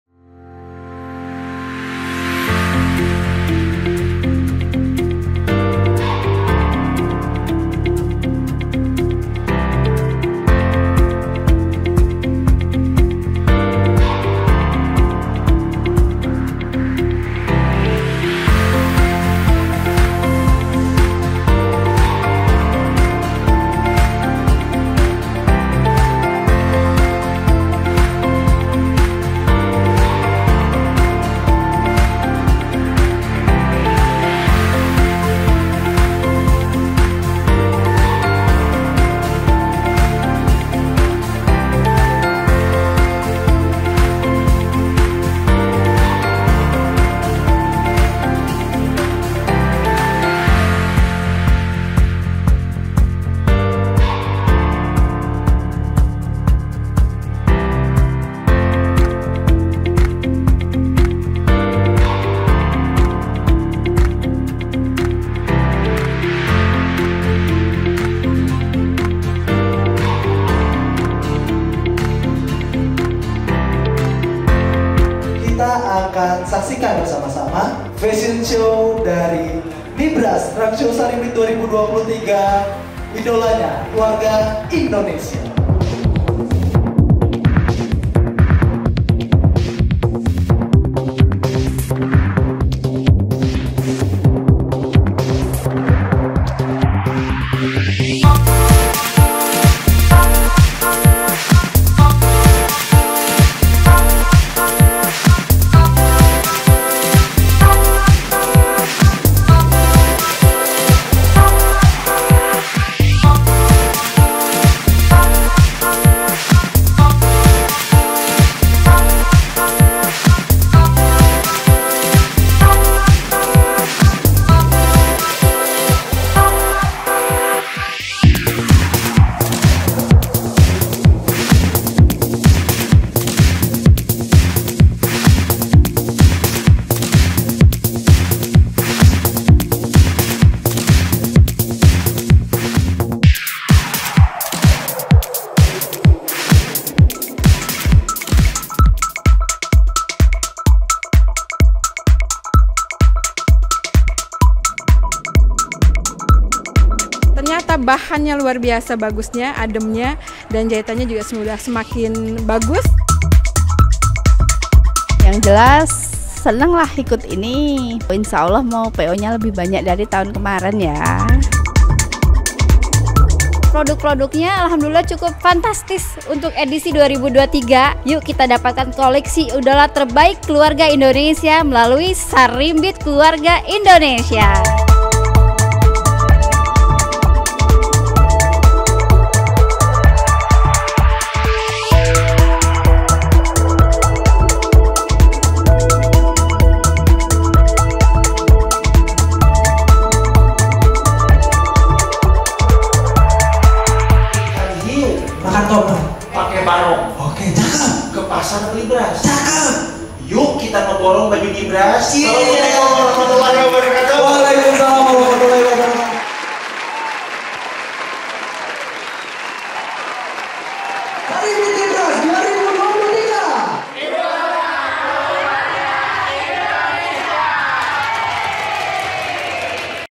NBRS Trunk Show Sarimbit Bandar Lampung 2023
kali ini Tim NBRS ada di kota Bandar Lampung dan kami ucapkan terimakasih atau sambutan dan kemeriahannya.

Berkat antusias paramitra dan semuanya. NBRS Trunk Show Sarimbit Bandar Lampung 2023 berhasil diadakan dengan lancar🙌🏻

Sampai ketemu lagi kota Bandar Lampung ✨